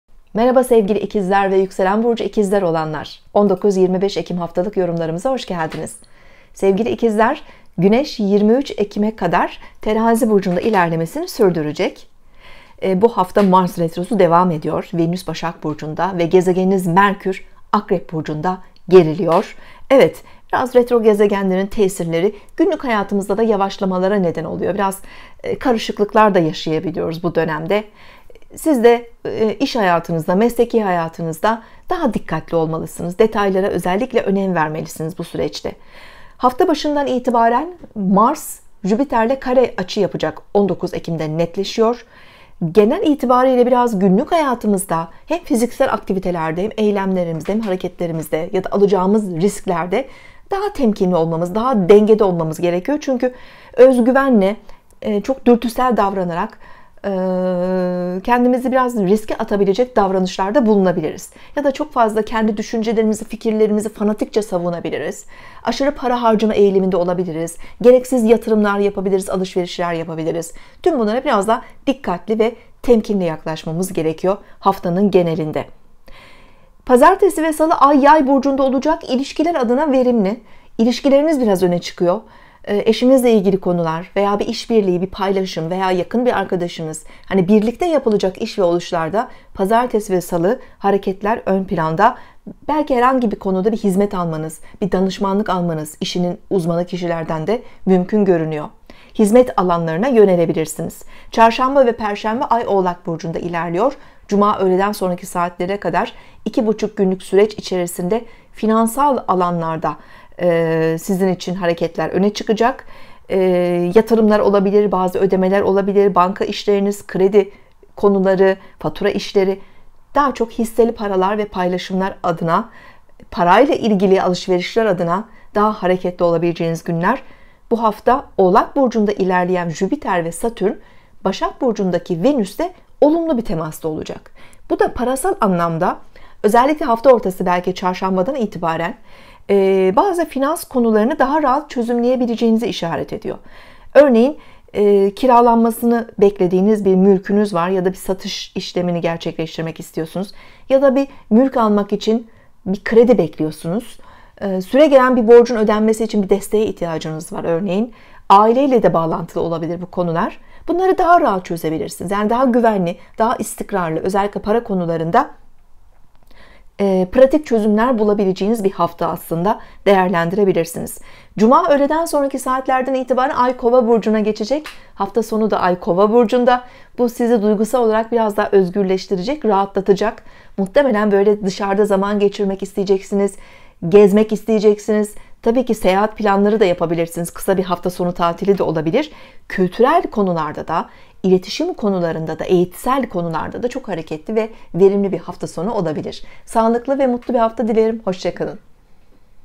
ikizler Burcu, Haftalık Burç Yorumları, 19-25 Ekim 2020 (Astroloji)
19-25 Ekim 2020 Haftalık Burç Yorumları (ikizler ve Yükselen ikizler Burcu)
ikizler ve Yükselen ikizler Burcu 19-25 Ekim Ekim haftası haftalık sağlık, iş, kariyer haftalık burç yorumları (Astroloji)